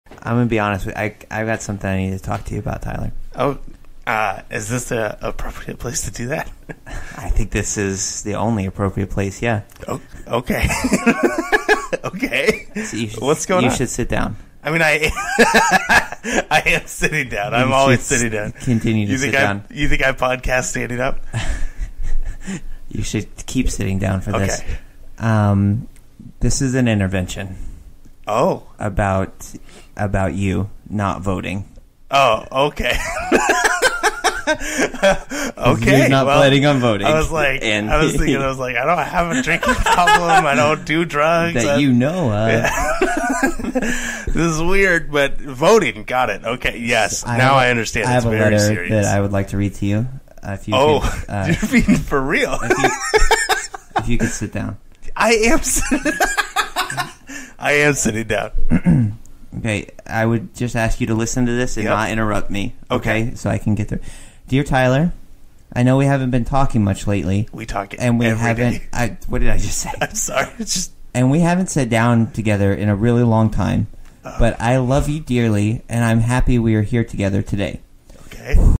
Teaser: The ElectionDay Intervention Episode
When it's ElectionDay and someone you love ISN'T planning to vote, what should you do? Why, hold an intervention, of course! Hear how it went, only on the Canby Now Podcast!